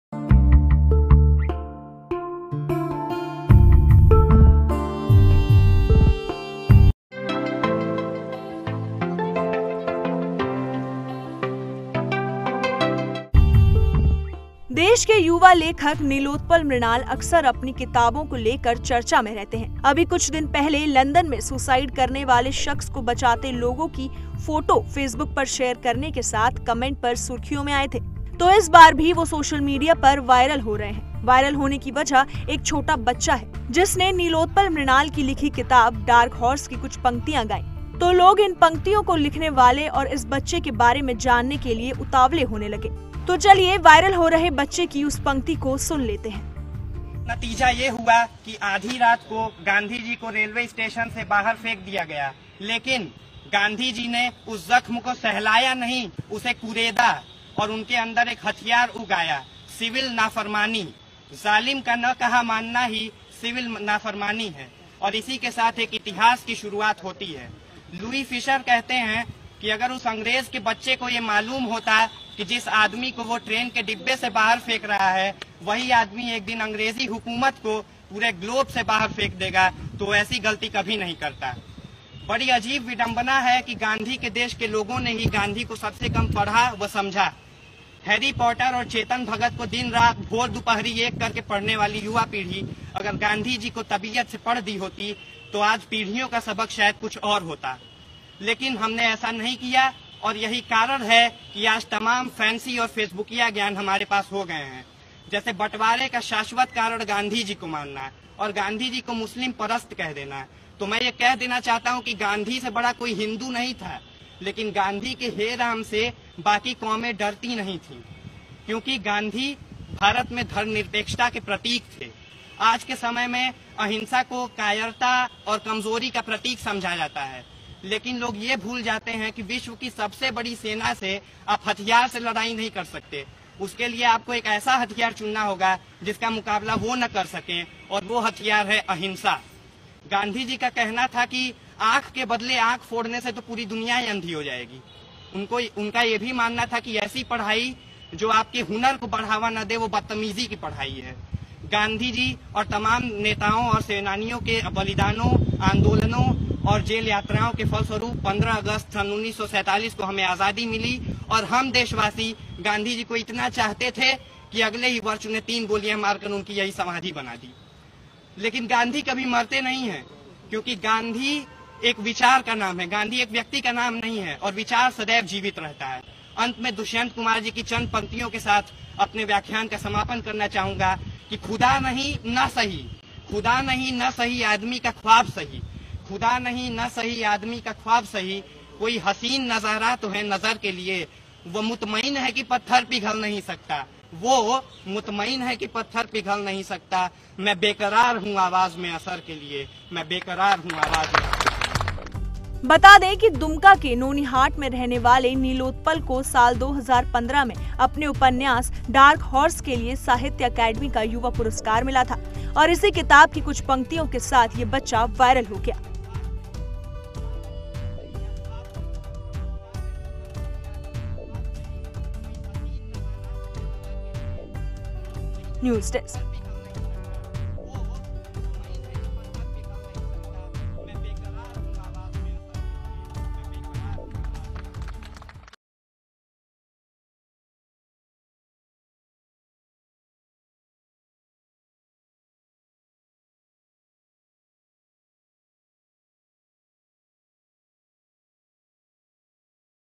जानिए कौन हैं Nilotpal Mrinal, जिनकी पंक्ति को पढ़कर ये लड़का वायरल हो गया
देश के युवा लेखक नीलोत्पल मृणाल अक्सर अपनी किताबों को लेकर चर्चा में रहते है...अभी कुछ दिन पहले लंदन में सुसाइड करने वाले शख्स को बचाते लोगों की फोटो फेसबुक पर शेयर करने के साथ कमेंट पर सुर्खियों में आए थे...तो इस बार भी वह सोशल मीडिया पर वायरल हो रहे है...वायरल होने की वजह है...एक छोटा बच्चा...